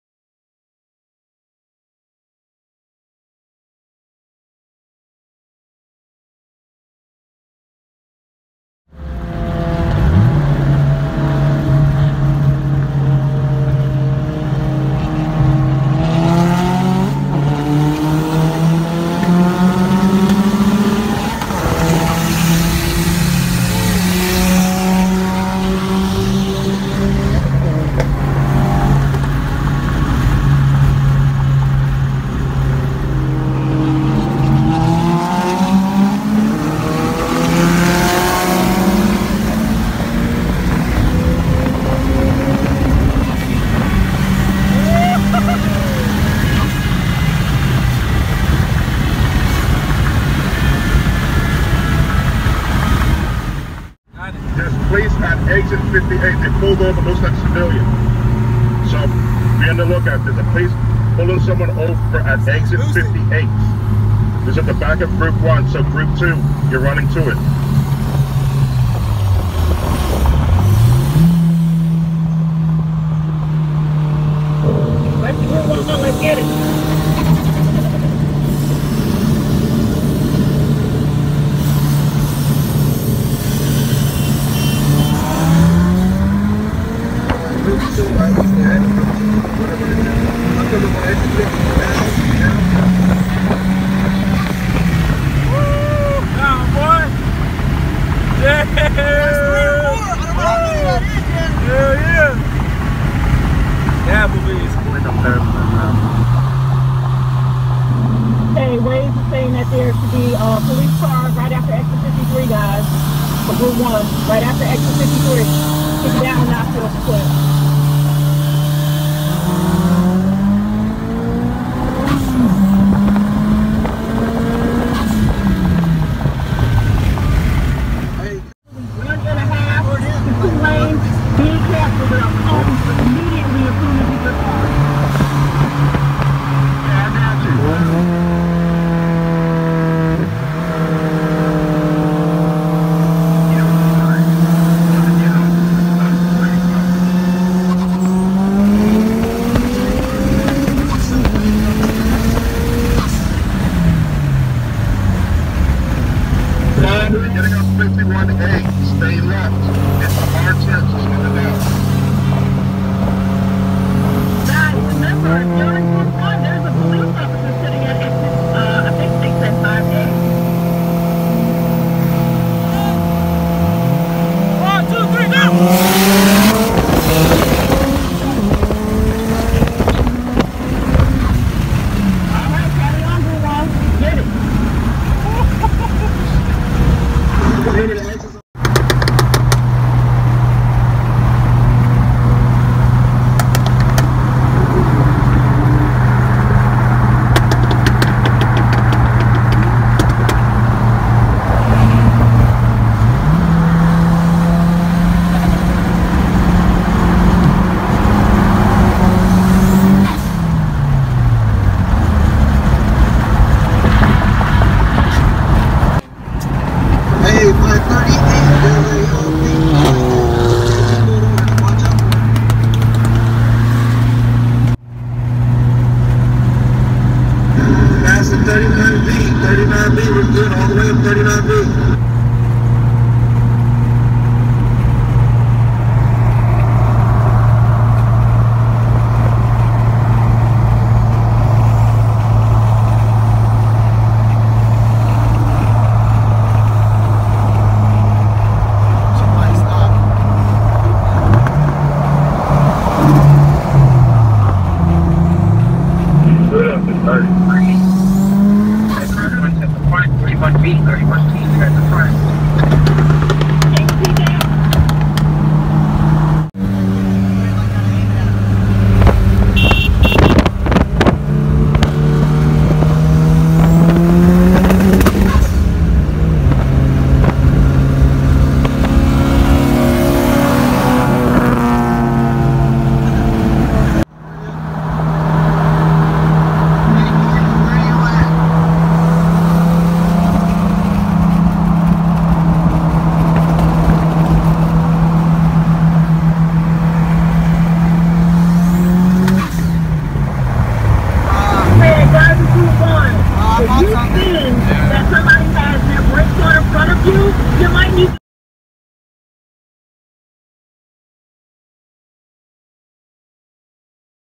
MC BUILT LANCER EVO 8 vs EVERYONE STREET RACE
Roll racing multiple cars with the EVO 8. Night was ended short with coolant hose popped off.